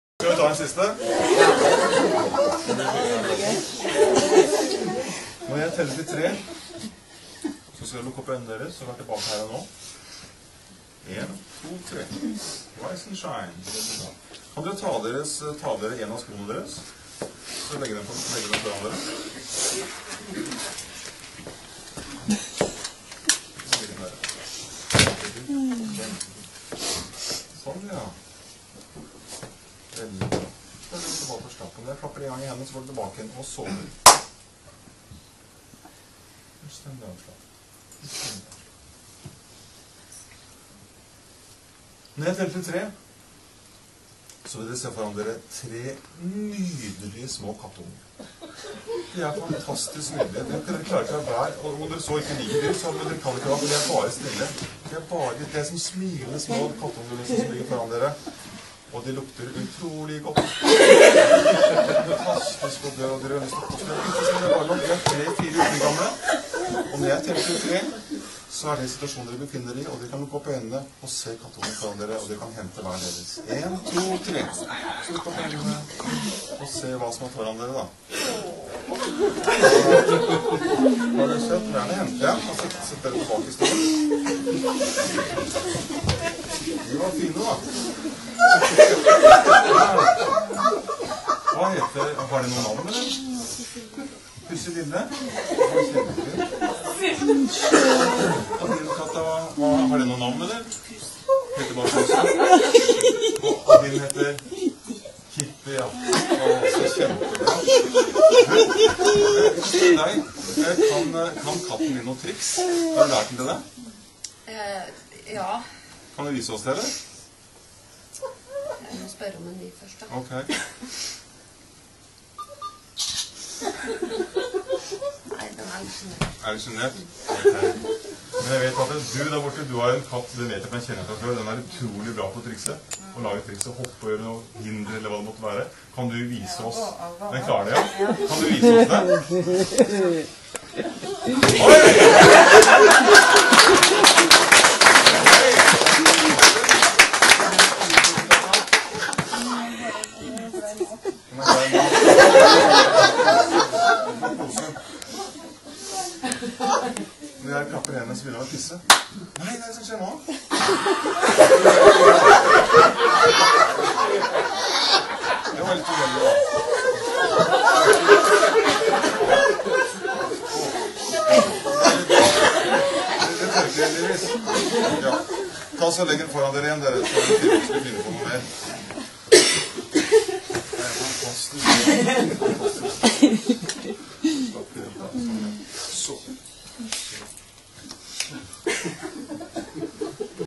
Hypnose kattunger
Fra hypnoseshow med Geir Tangen på Tante Theoline i Skien.
En sko er plutselig en søt kattunge ;)